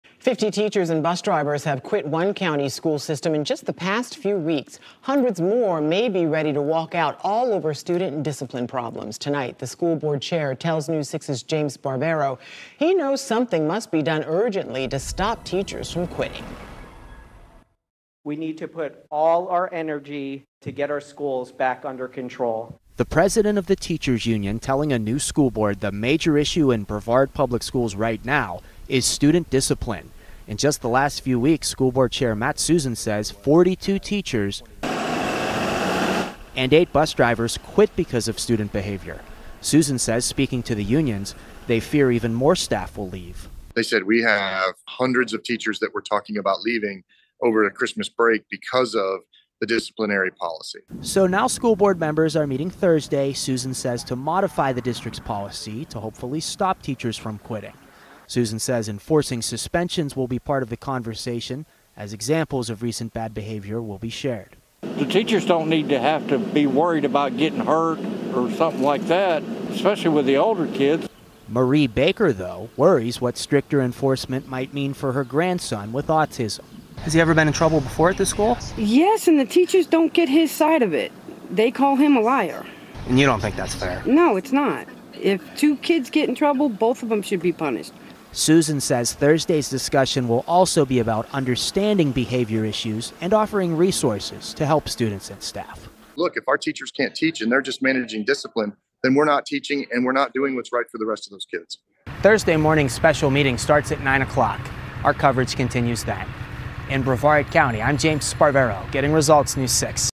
Debate over student discipline continues in Brevard County
In just the last few weeks, Brevard County School Board Chair Matt Susin said 42 teachers and eight bus drivers have quit because of student behavior.